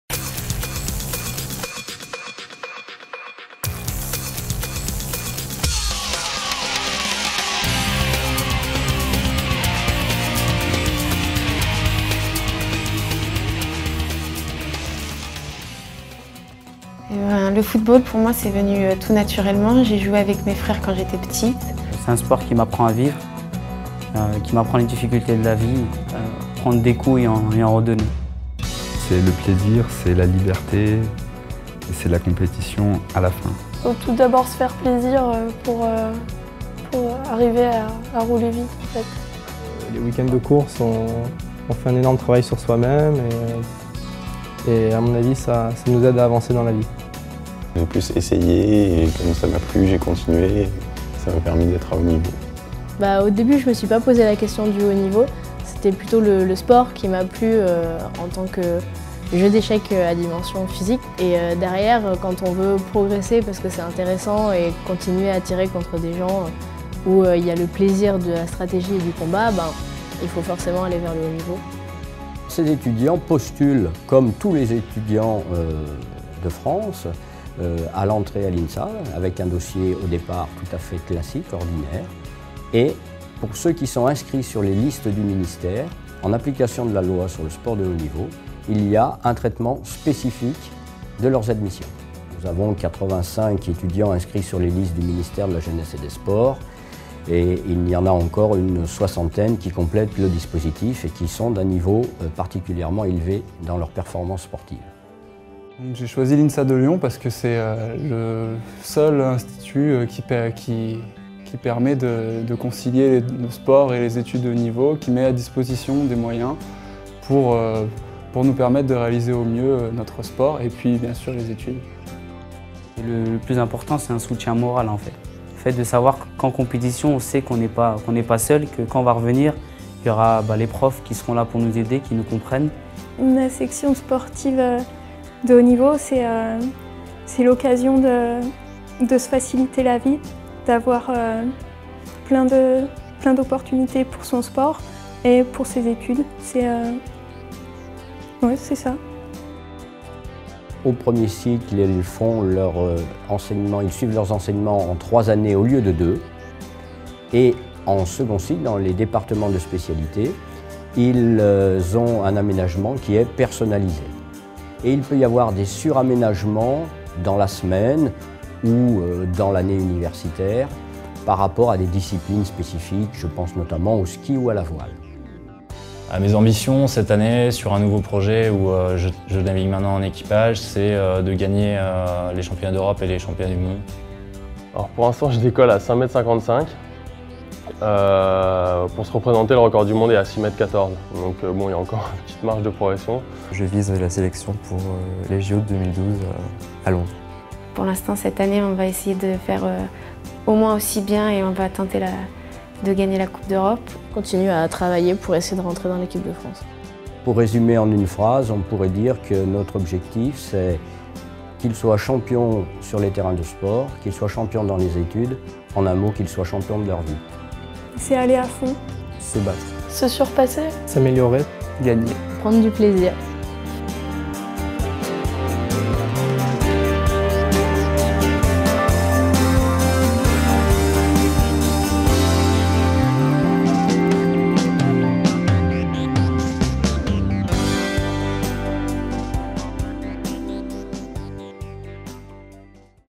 La Section Sport de Haut Niveau de l'INSA de Lyon
La SSHN a pour objectif de permettre à des jeunes sportifs de qualité inscrits sur les listes ministérielles, de mener de front la poursuite de leur carrière sportive et la préparation d'un diplôme d'ingénieur, grâce à l'aménagement de leur emploi du temps et l'allongement de leur cursus. Elle peut également servir de structure, d'"interface" entre les nombreux laboratoires de l'école et les intervenants dans la recherche appliquée au sport.